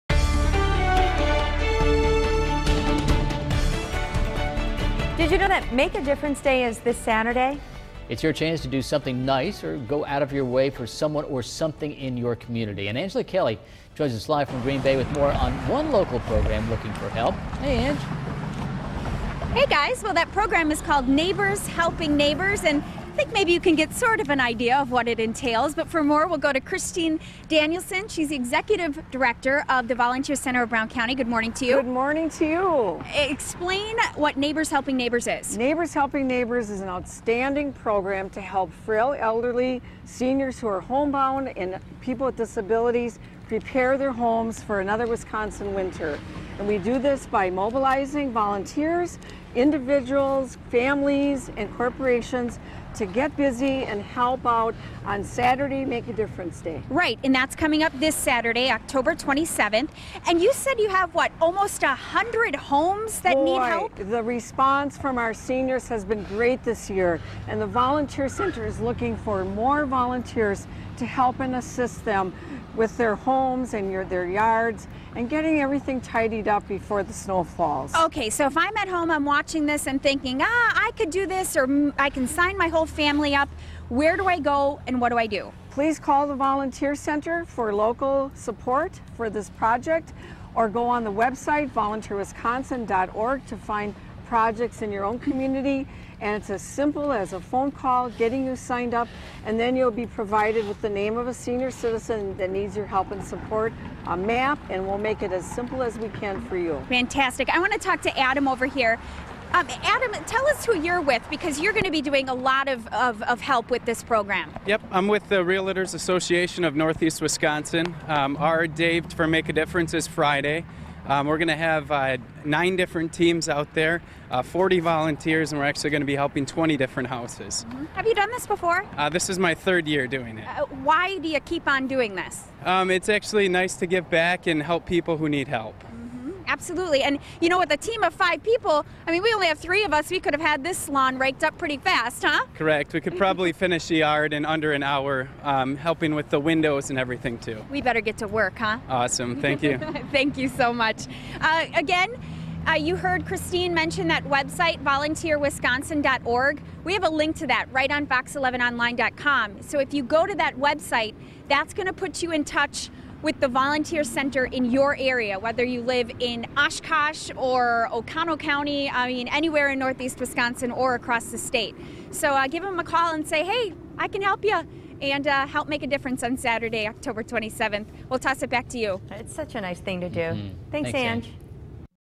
GDW 7 Making a Difference Day
Find out how to help out on Making a Difference Day.